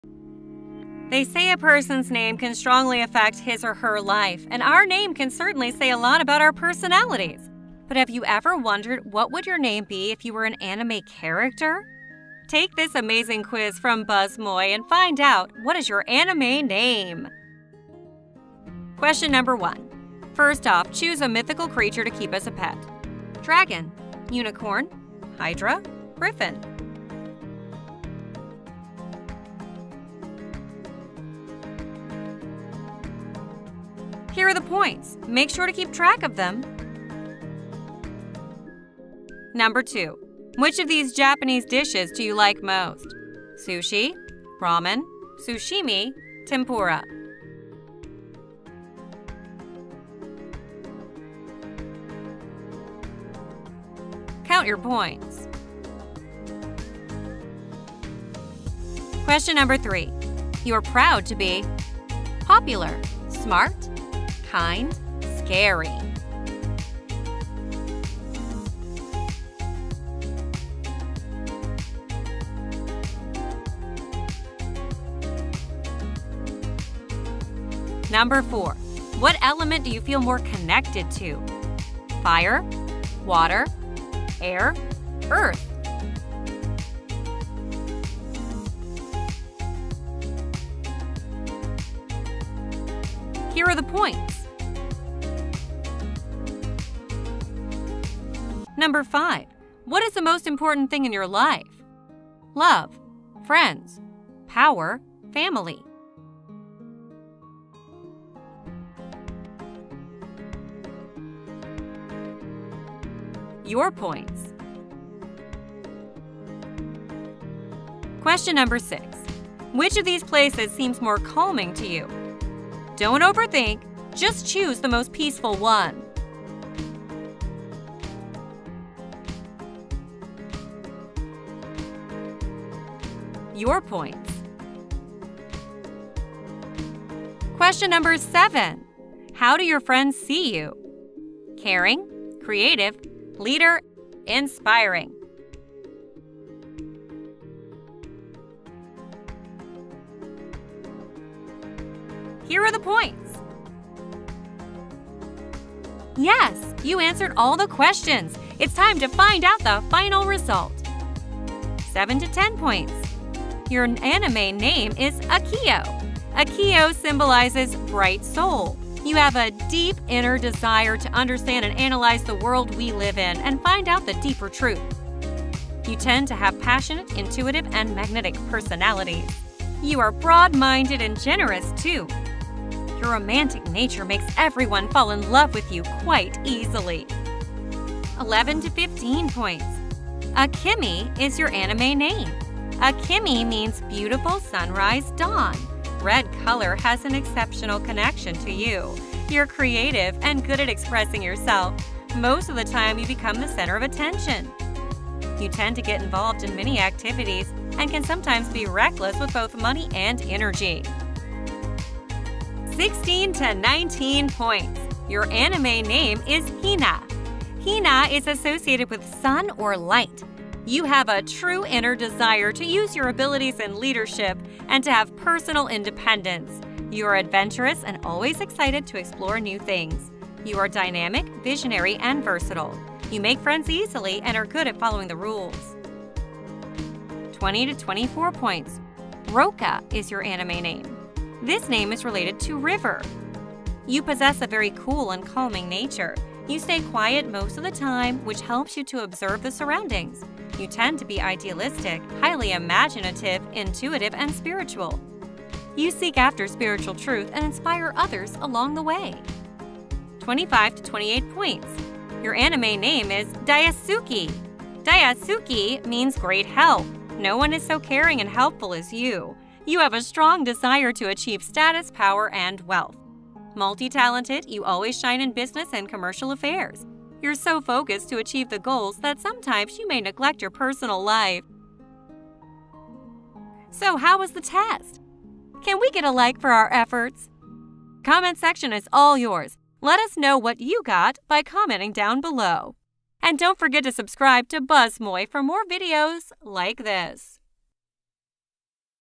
What Is Your Anime Name?
They say a person’s name can strongly affect his or her life. And our name can certainly say a lot about our personalities. But have you ever wondered what would your name be if you were an anime character?

Take this amazing quiz from BuzzMoy and find out ‘What is your Anime name?’